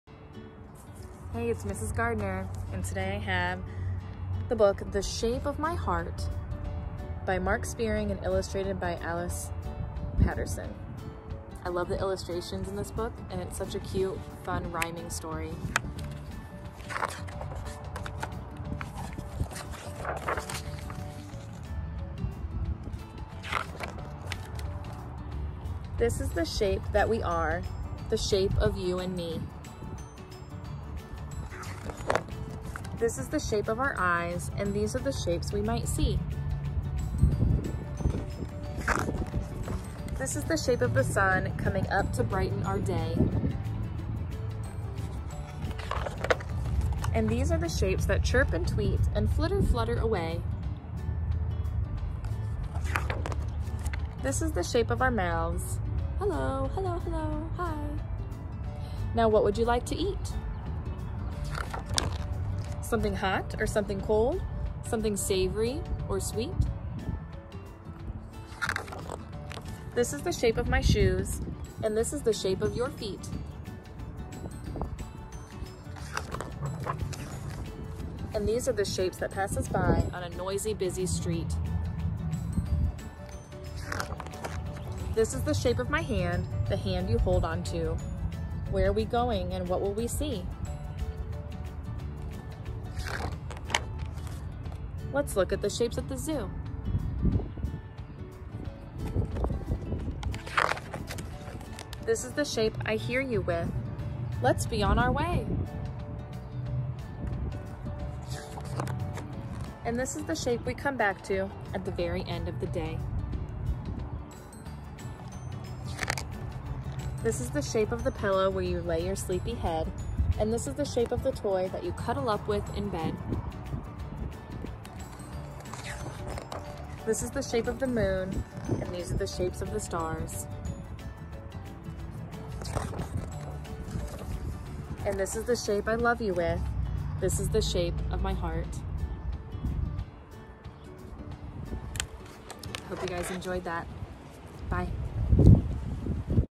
Mrs. Gardner Reads The Shape of My Heart
A cute and fun book written by Mark Sperring and illustrated by Alys Paterson. Its a sweet rhyming story with fun pictures with lots of shapes to find and discover. Hope you enjoy! This would make the perfect bedtime story to snuggle up with too. 🥰